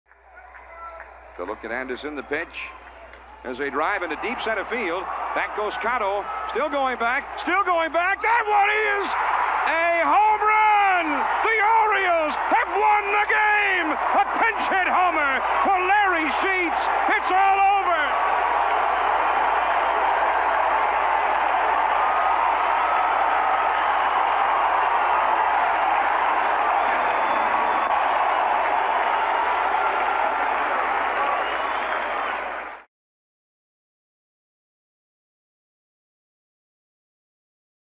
Sheets slugs pinch-hit walk-off home run
8/24/88: In Game 1 of a doubleheader, Larry Sheets blasts a pinch-hit, two-run home run with two outs to give the Orioles a 4-3 walk-off win